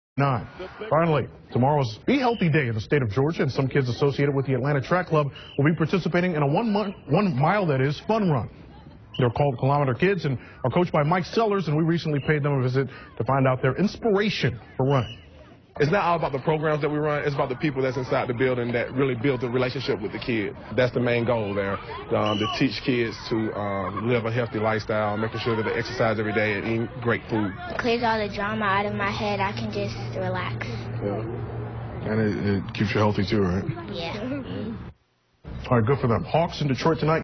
Kilometer Kids- Track (Atlanta)
Coach Mike Sellers talks to Channel 2 News about Kilometer Kids Track Program